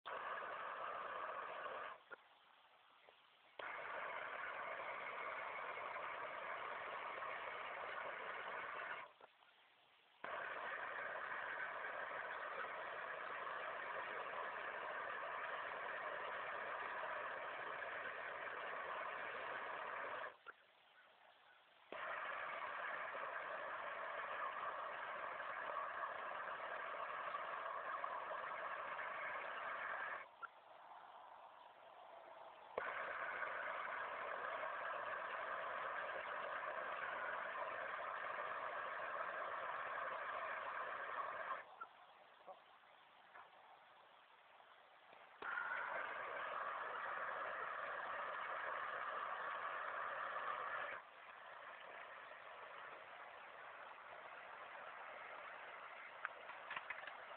Thermite
Golem burns a pc with thermite.